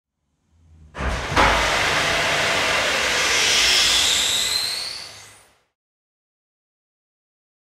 Delta Force F-14 Jet Idle and Catapult Take Off
Delta Force F-14 Jet Idle and Catapult Take Off · Soud Ideas · Traditional

G.I. Joe Sound Effects